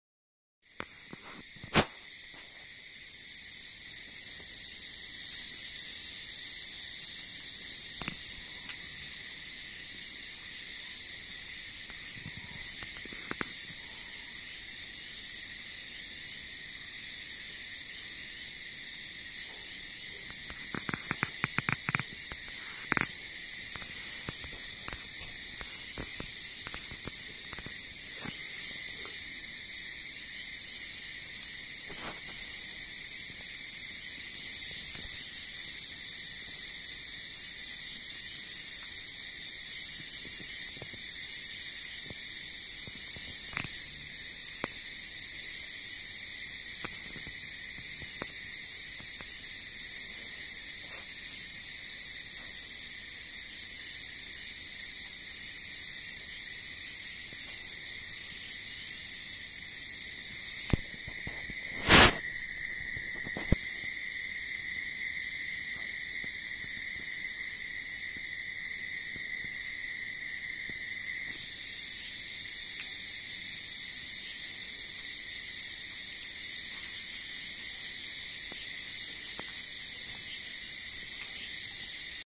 LUNAR ECLIPSE 2017 August 7
Partial Lunar eclipse happened in India on August 7 2017 from 22:52 to 00:48